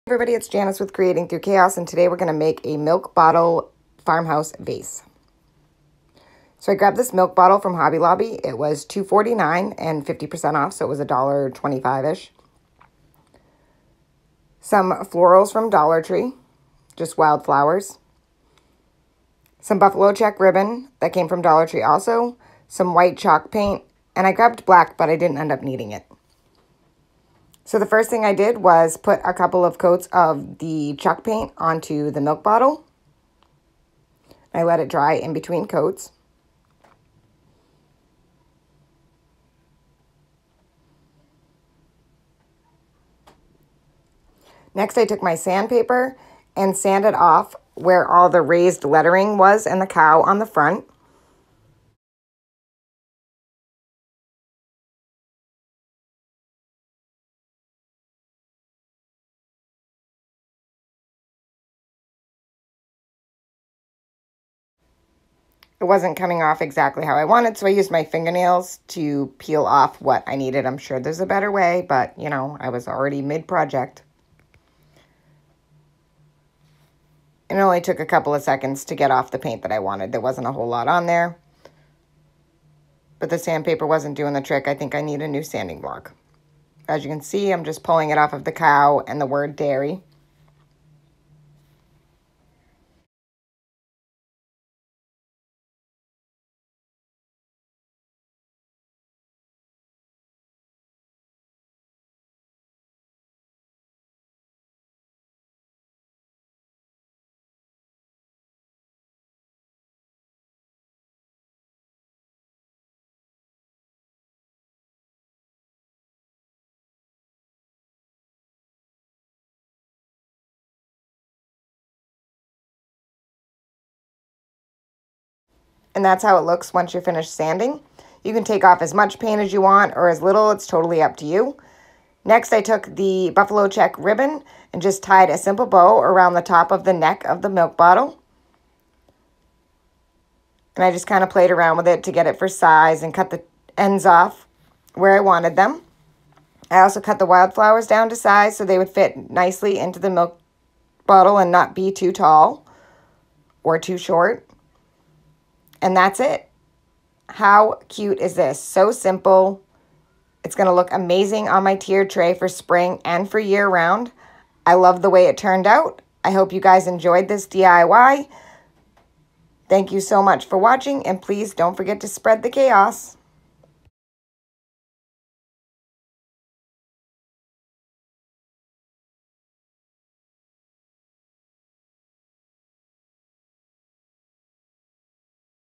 DIY Farmhouse Mille Bottle Vase Tier Tray Decor
A super easy DIY Farmhouse Milk Bottle Vase! A great addition to your tier tray and farmhouse decor! So simple and cute! I love how it turned out!

If you enjoyed this DIY, come find me on my Blog and Facebook for more DIY’s!